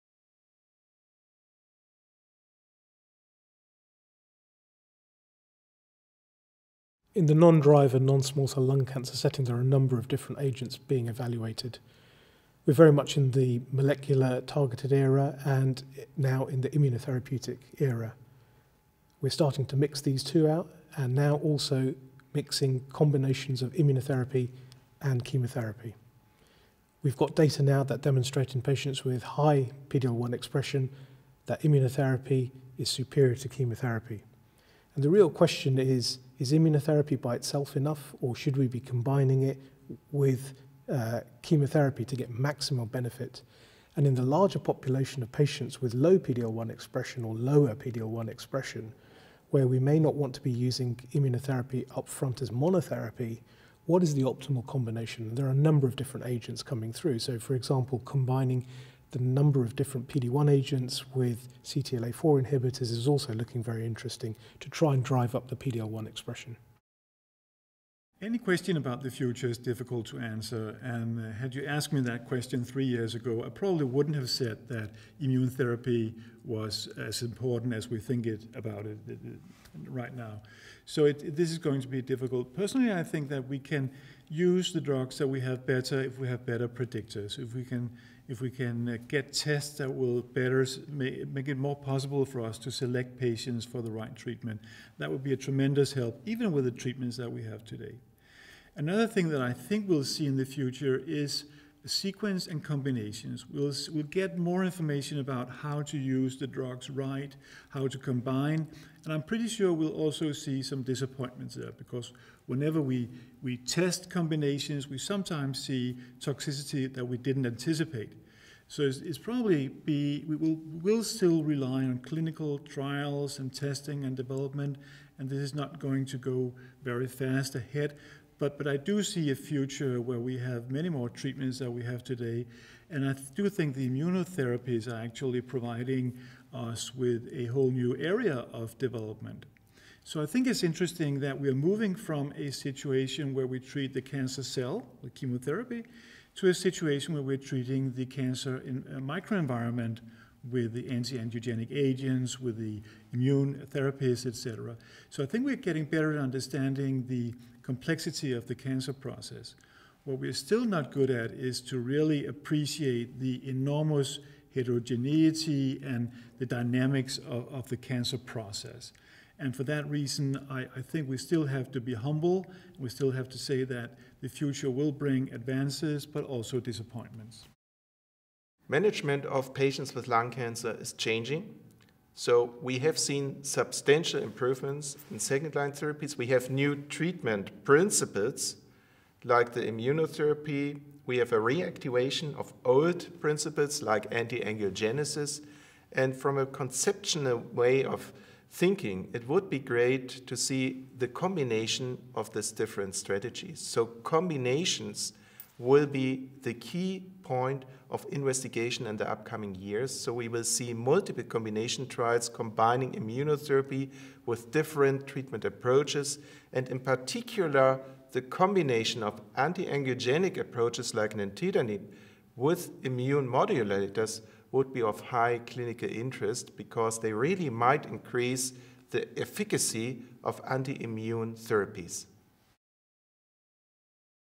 The Future of Non-Driver Lung Adenocarcinoma
Sanjay Popat, PhD; Anders Mellemgaard, MD; and Martin Reck, MD, PhD, discuss potential strategies for the future management of non-driver lung adenocarcinoma.